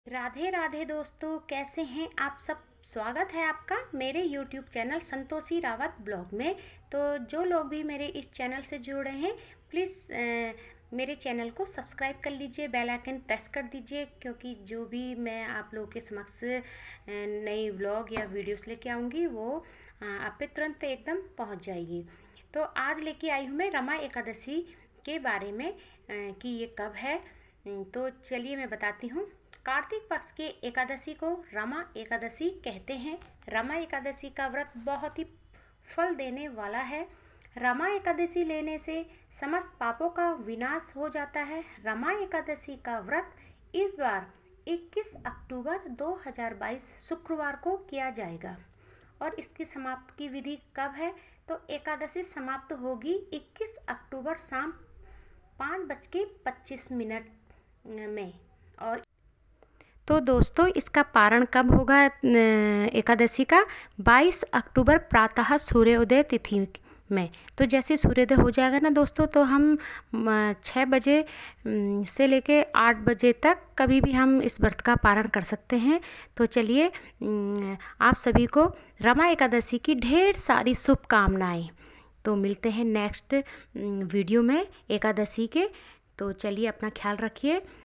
एकादशी कब है/कार्तिक कृष्ण पक्ष रमा एकादशी/ ekadashi Kab hai /
Radhe Radhe kaise hain aap sab swagat hai aapka Mere channel mein 💕
Today I m going to Show you How to take rama ekdashi vrat💞,stay active for more upcoming Vlogs !